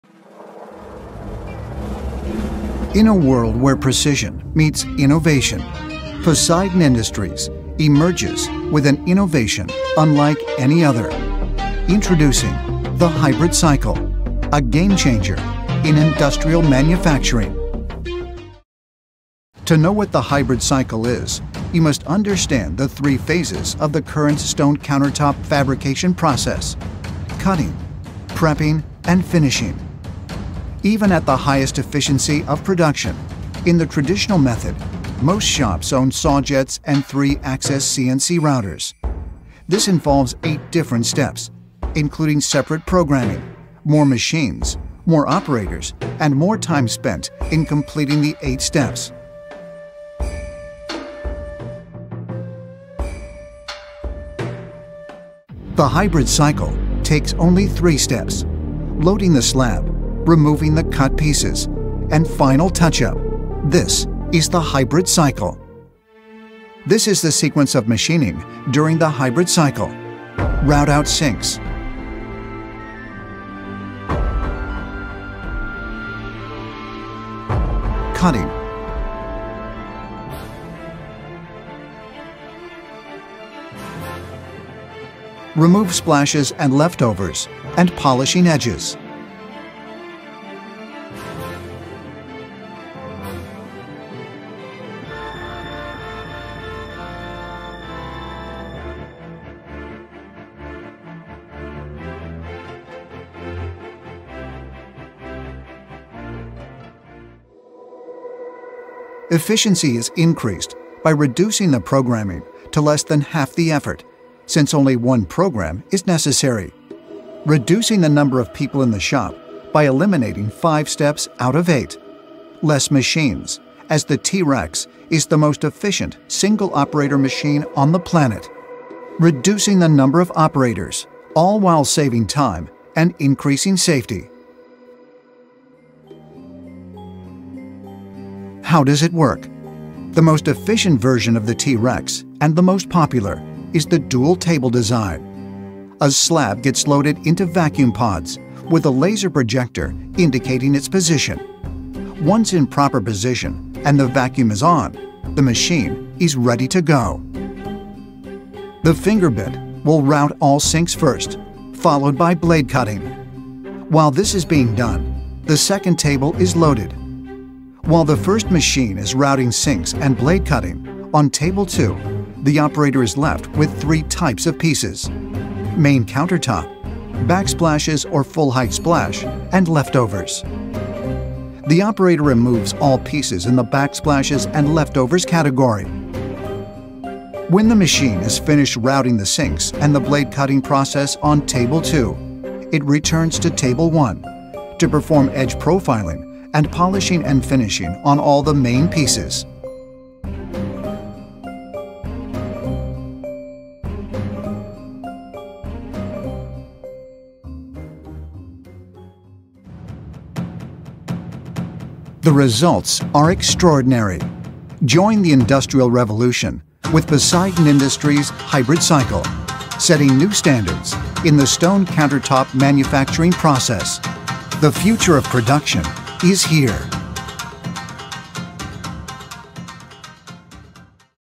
What is the Hybrid Cycle? ™️
What is the Hybrid Cycle тм? Short Answer: It is the fastest way in the world to fabricate kitchen countertops!

Message us to learn more!